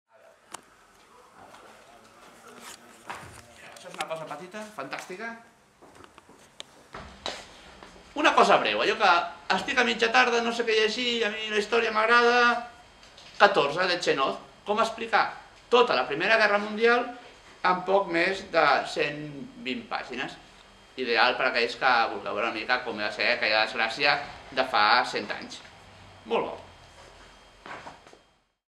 Dia 4 del gran confinament. Avui una cosa breu però intensa: 14 de Jean Echenoz!
Com explicar el que va significar la primera Guerra Mundial en poc més de 100 pàgines. És molt interessant rellegir-ho en el confinament. En la traducció d' Anna Casasses i Javier Albiñana Serraín, publicat per Editorial Anagrama i Raig Verd Editorial . Lectura breu que no lleugera que ens acompanya i ens ensenya a mirar el món d'una manera diferent. Com la nostra realitat.